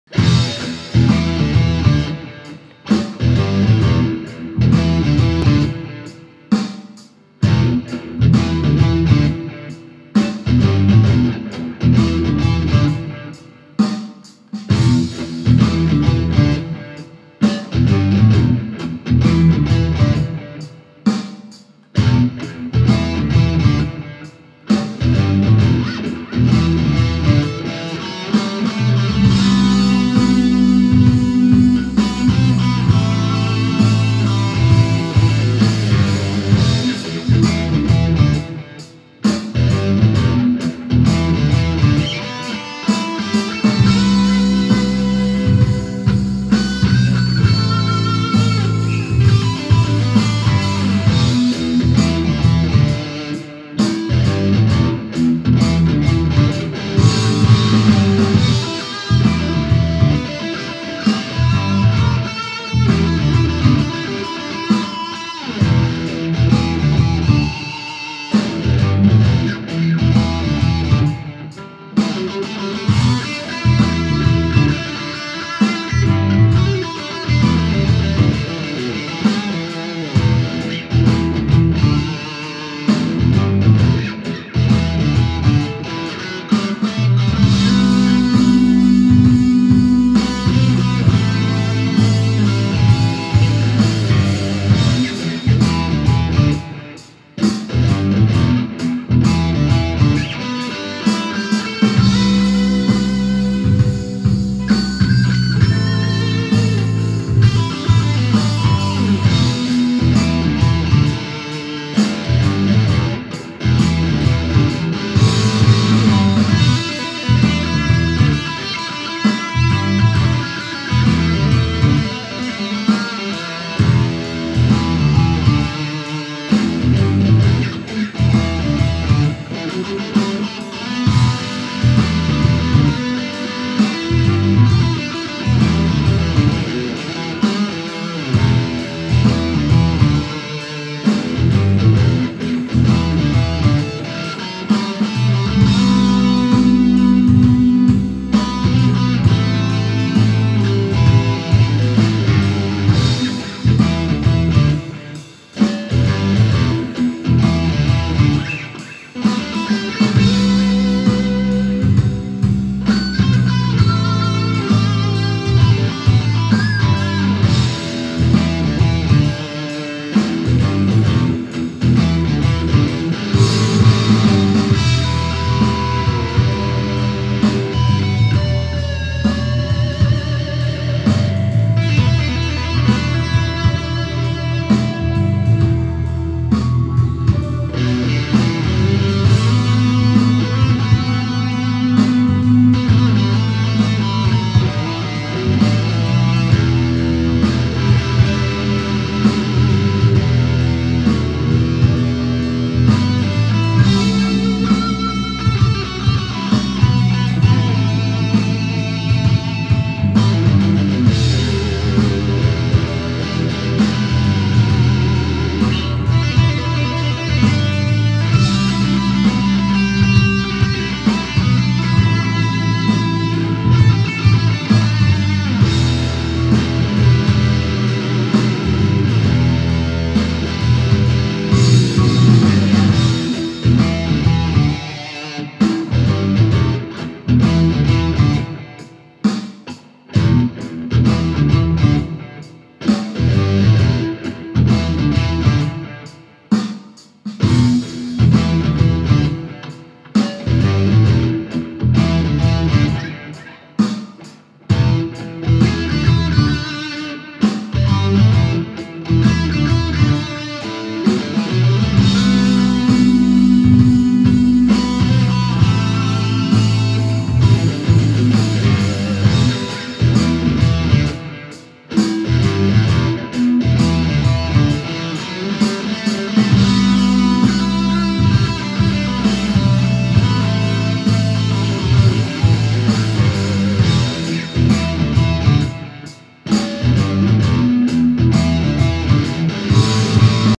gg blues rock jam g and l legacy usa
jamming over the top of a youtube guthrie govan backing track on my g and l legacy one of the older 96 usa ones with 3 bolt neck